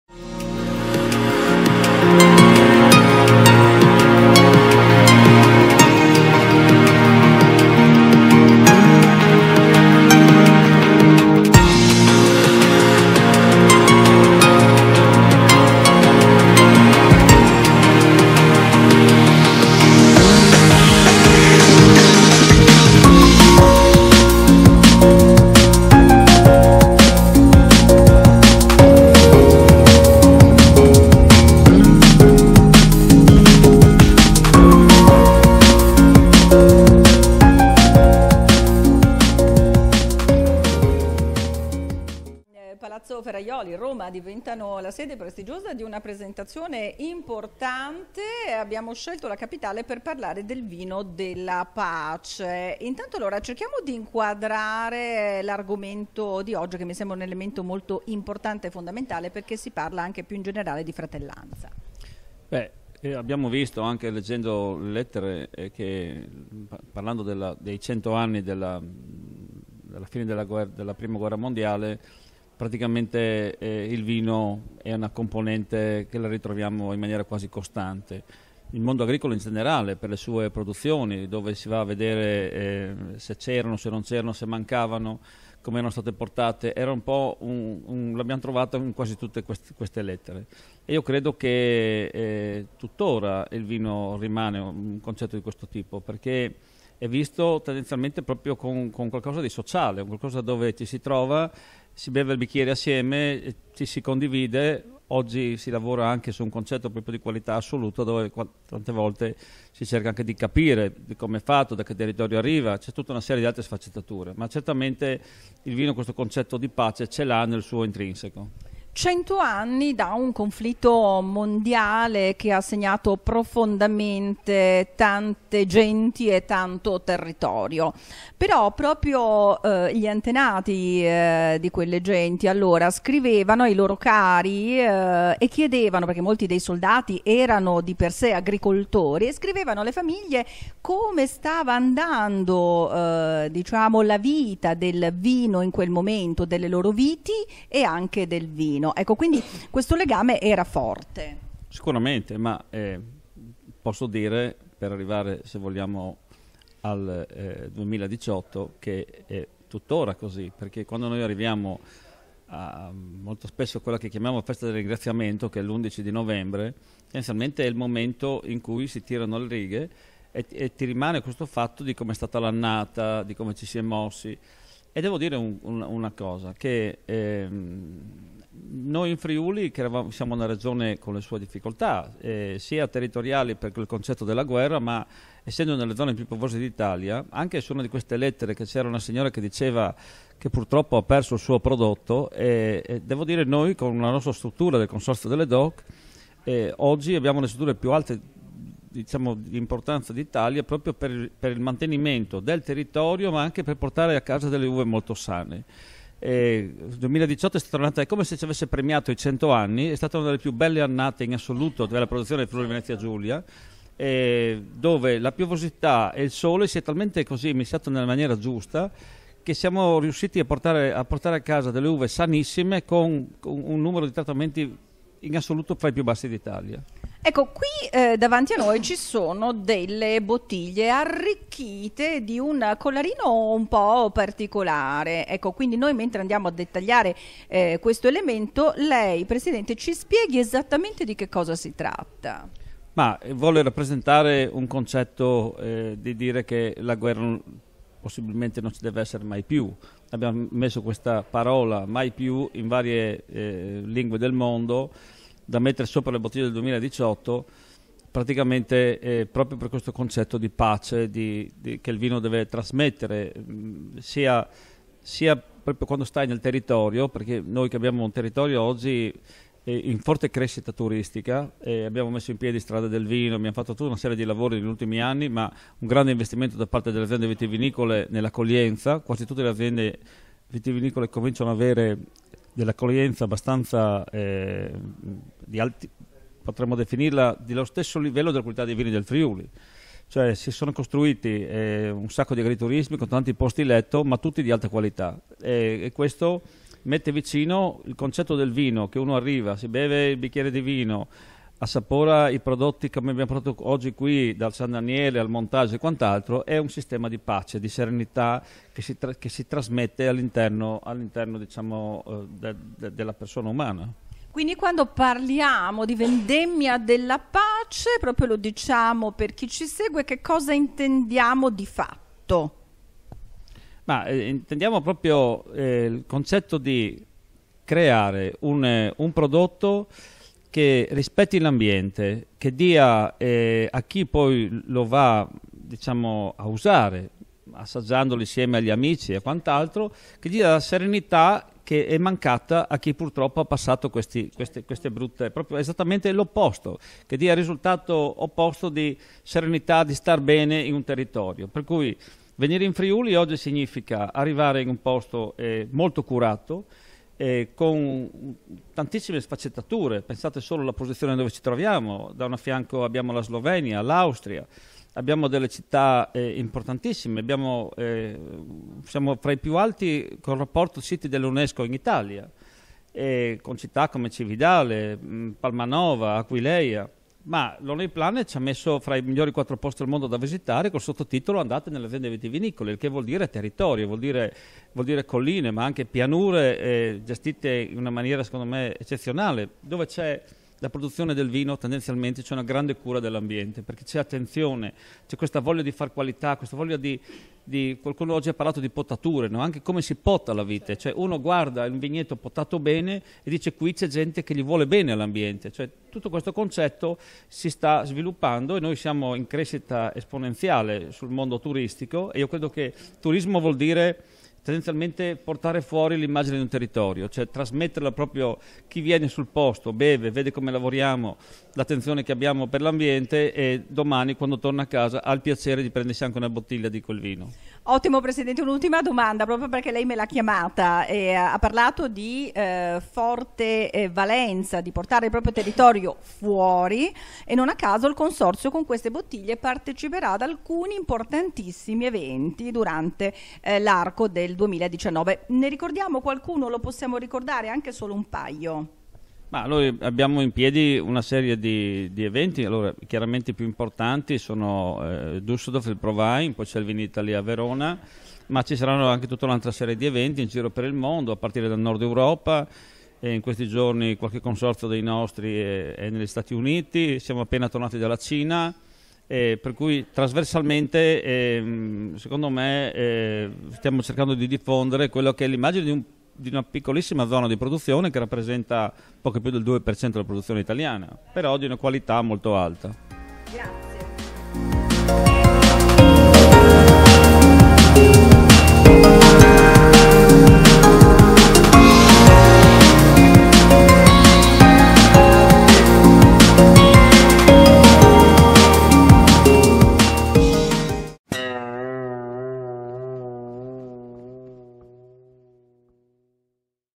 Adriano Gigante, Presidente del Consorzio DOG - FVG: "1918 - 2018 una vendemmia di Pace"
Intervista di Camilla Nata ad Adriano Gigante, Presidente del Consorzio DOG - FVG, a margine della presentazione dell'inziativa "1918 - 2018 una vendemmia di Pace" organizzata da Consorzio DOC - FVG.